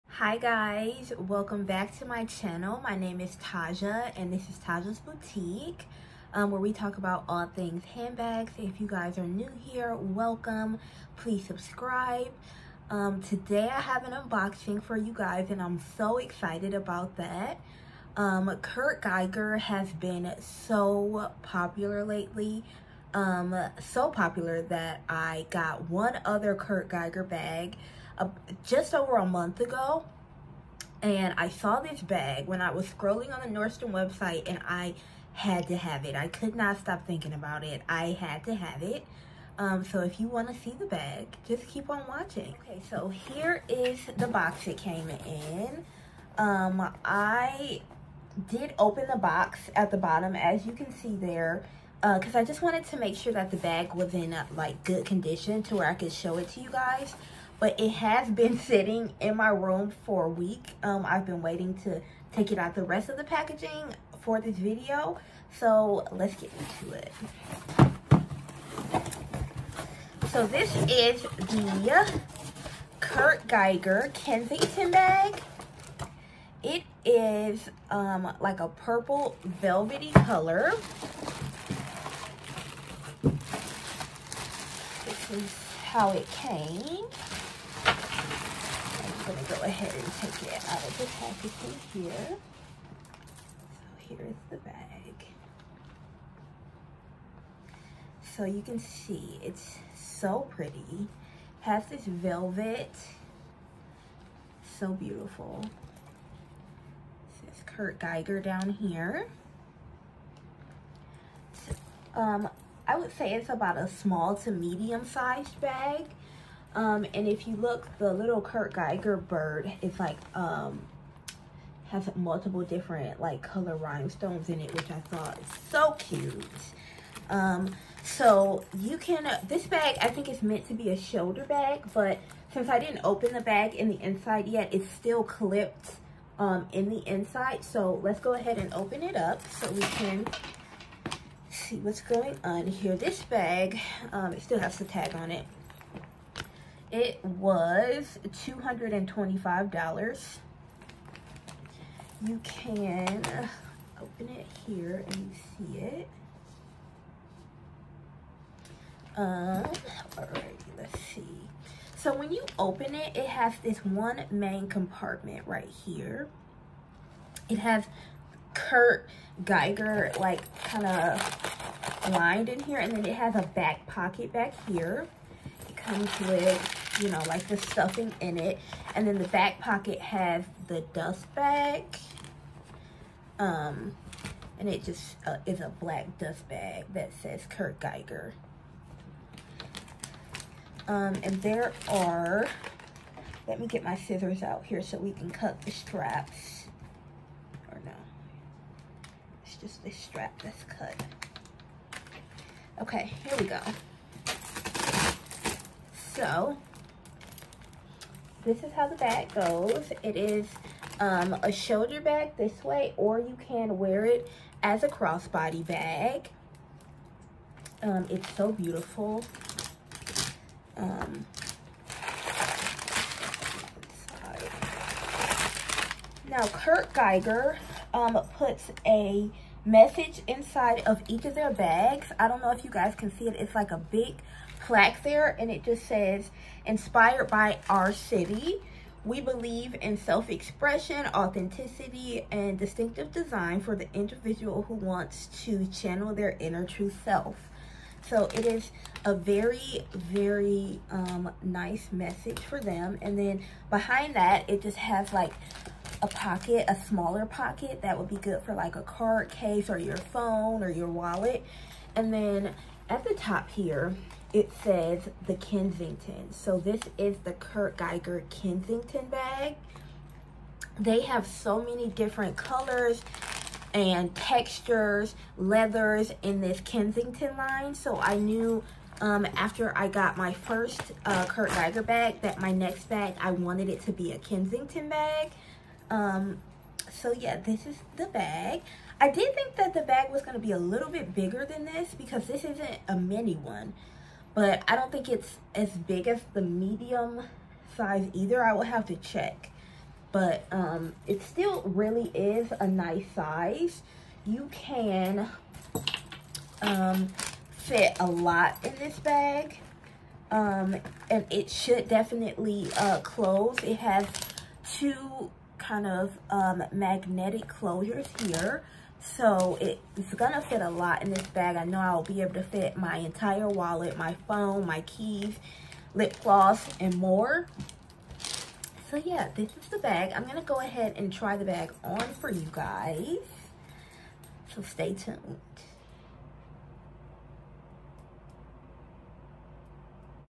Kurt Geiger Kensington Velvet Shoulder Bag Unboxing | Taja’s Boutique
• KURT GEIGER KENSINGTON VELVET SHOULDER BAG UNBOXING •

Hey yall!!! Welcome to Taja’s Boutique! I’m back with another video!!! This time it is a Kurt Geiger unboxing! I got this beautiful piece from nordstrom and I am excited to share her with you all!

Unfortunately I went back to the nordstrom website and the bag is sold out at the moment :(

Also, very sorry about the video quality! I am working to fix it!

• FOLLOW ME •